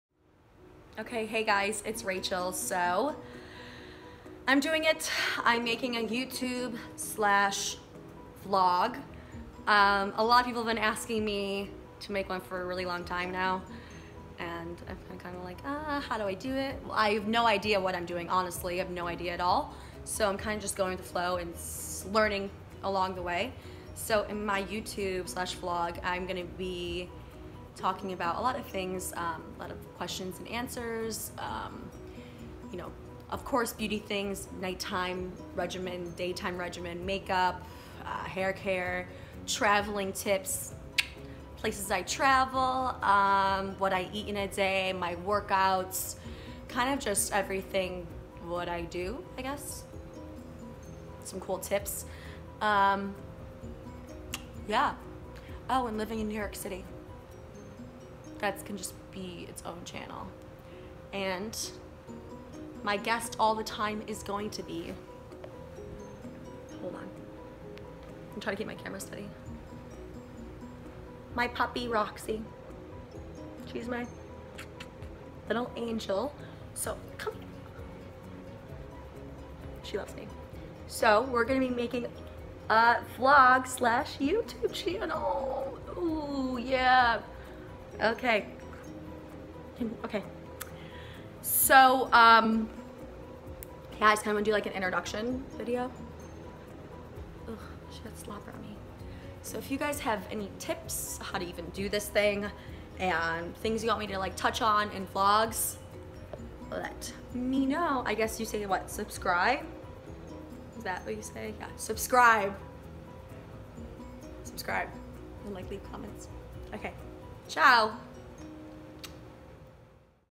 Rachel Hilbert: INTRO VIDEO to my vlog!!
Hey guys! So its here!... Here is a little intro of what i am going to be doing on my vlog! Im super new to this so here it goes! let me know if you have any tips/suggestions! xoxo